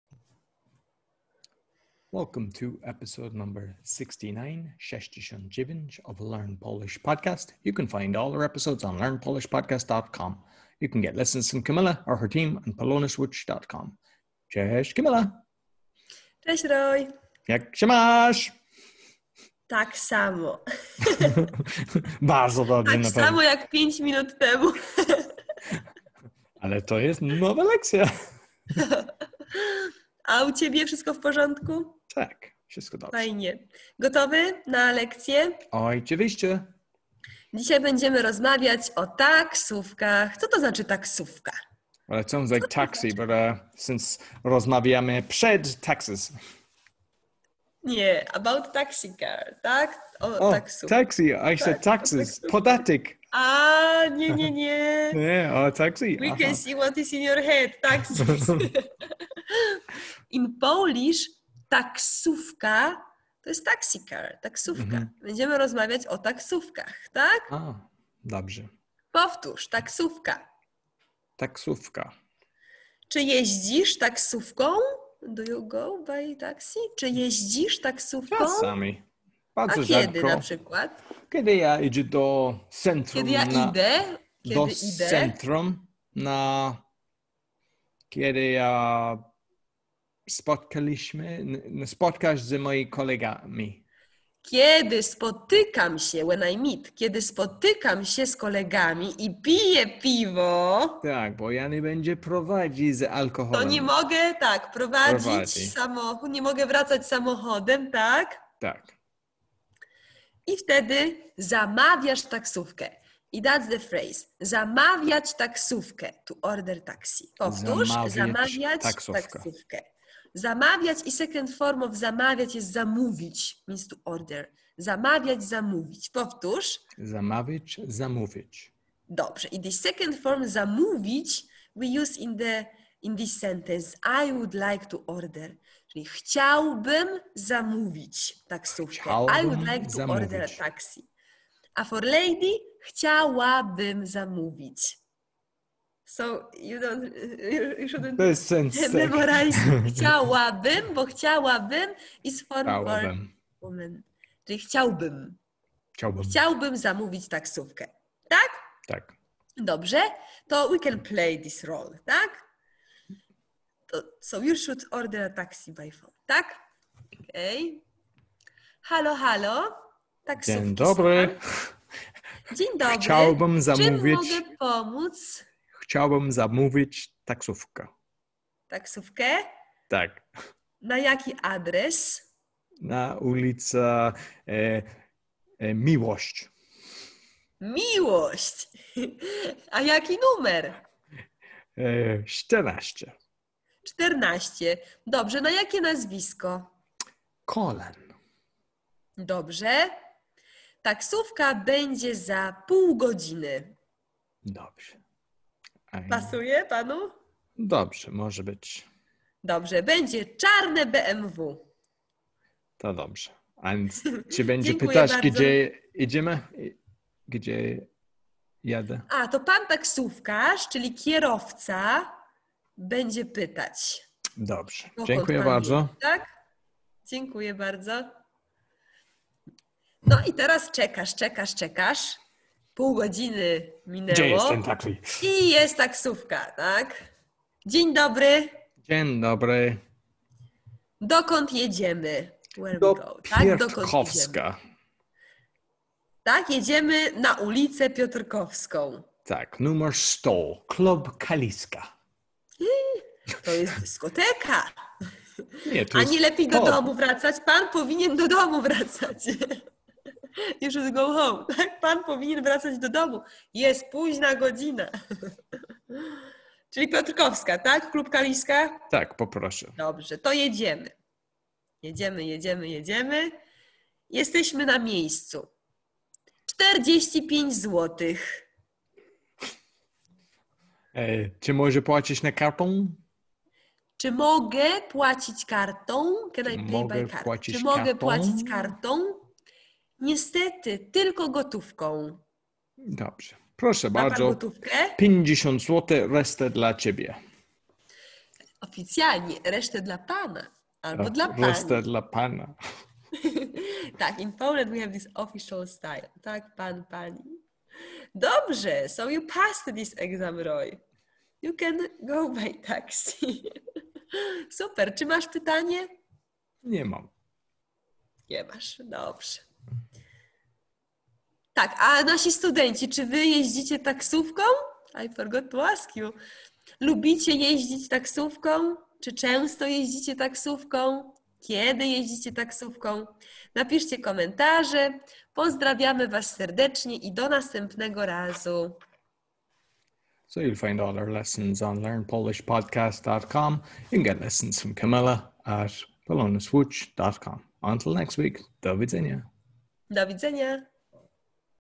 Learn Polish Podcast #69 Ordering a Taxi
To get Skype lessons from Kamila or her team please visits

In this Episode we discuss:

Taksówka- Taxi

Rozmawiamy o taksówkach- We are talking about taxis

Czy jeździsz taksówką?- Do you take a taxi?

Kiedy spotykam się z kolegami i pije piwo to nie mogę wracać samochodem- When I meet my friends and drink beer, I can't go back by car

Zamawiać (zamówić) taksówkę- To order a taxi

Chciałbym zamówić taksówkę- I would like to order a taxi

Chciałabym zamówić taksówkę- I would like to order a taxi

Dzień dobry, w czym mogę pomóc?- Good morning, how can I help you?

Na jaki adres?- To what address?

Jaki numer?- What number?

Na jakie nazwisko?- What surname?

Taksówka będzie za pół godziny- The taxi will be here in half an hour

Kierowca taksówki- Taxi driver

Dokąd jedziemy?- Where are we going?

Jedziemy na ulicę Piotrkowską- We're going to Piotrkowska Street

Jesteśmy na miejscu- We are there

Czy mogę płacić kartą?- Can I pay by card?

Niestety, tylko gotówką- Unfortunately, only in cash

Reszta dla Pana- Keep the change (the change is for you)

Czy lubicie jeździć taksówką?- Do you like to take a taxi?

Czy często jeździcie taksówką?- Do you often take a taxi?

All Polish Episodes / Speaking Podcast / Meditation Podcast / Awakening Podcast/ Polish Property & business Offers